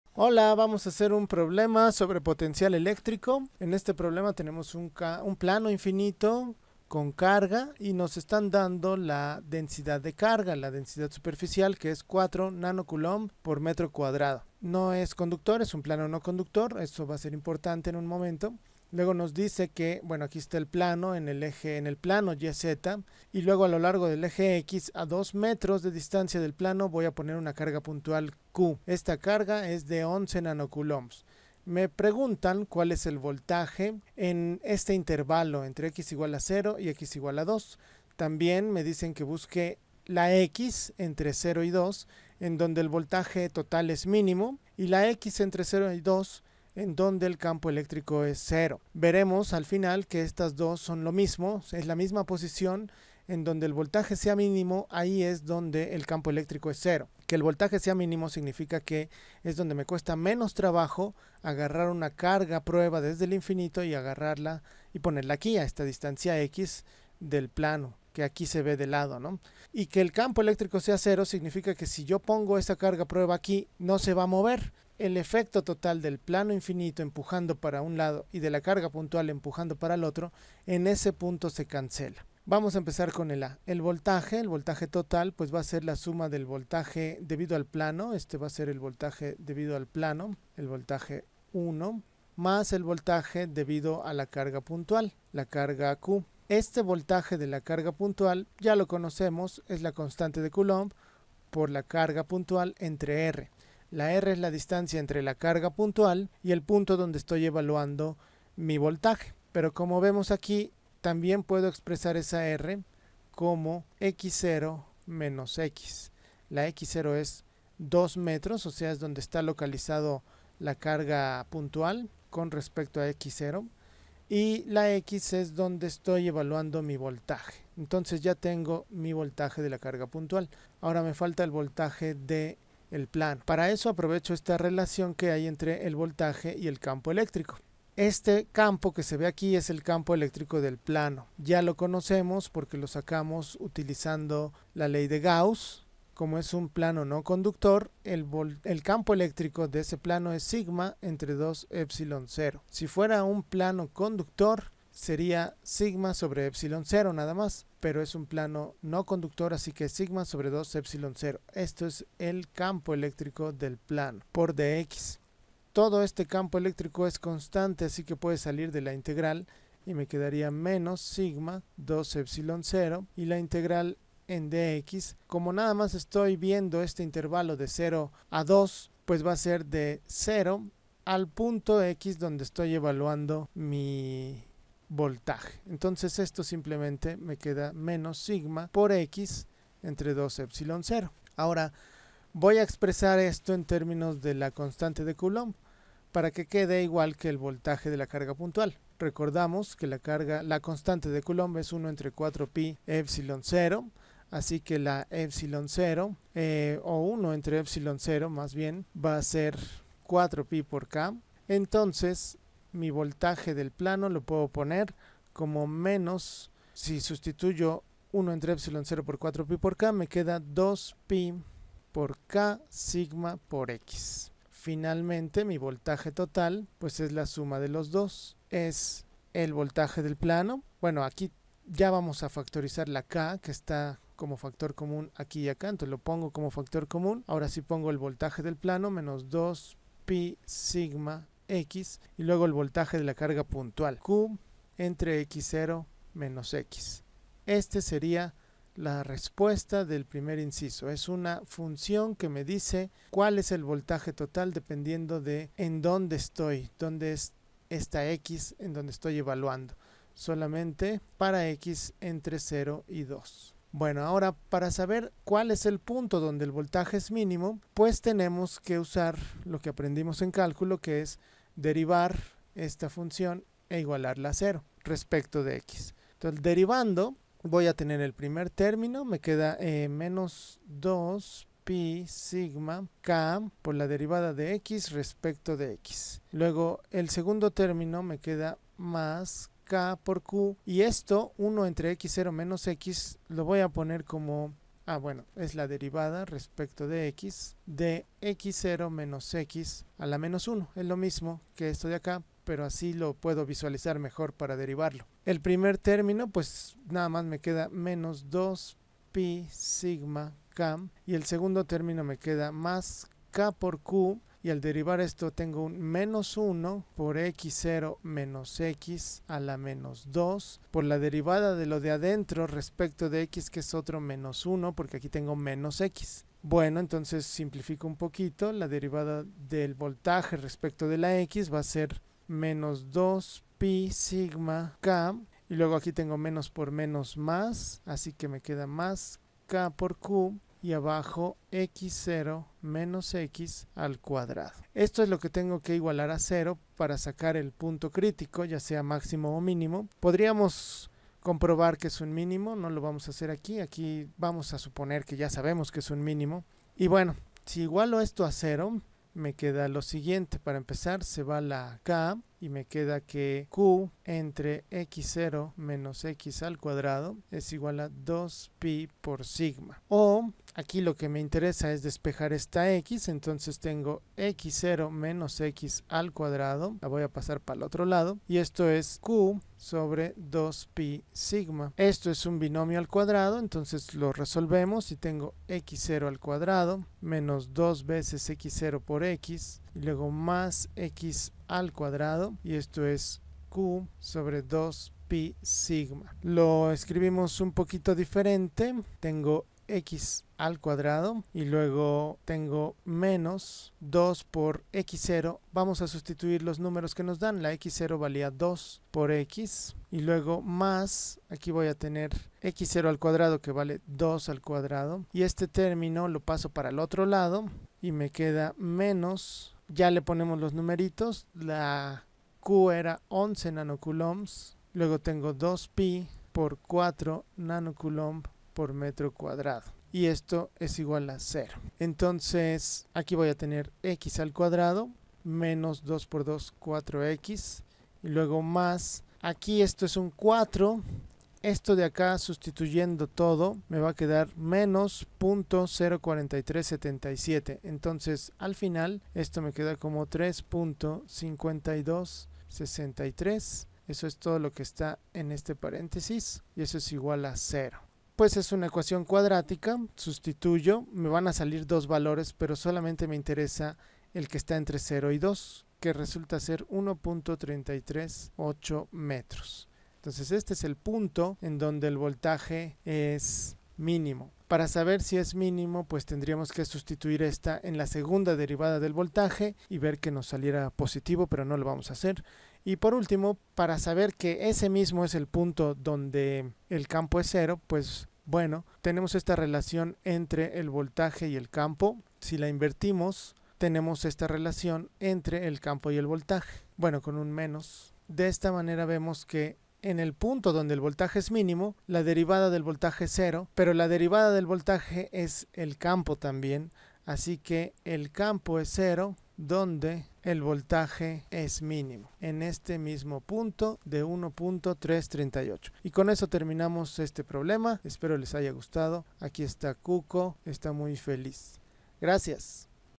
Electricidad y Magnetismo - Unidad 04 - Problema resuelto de Potencial Eléctrico
Voltaje debido a un plano infinito con carga y una carga puntual.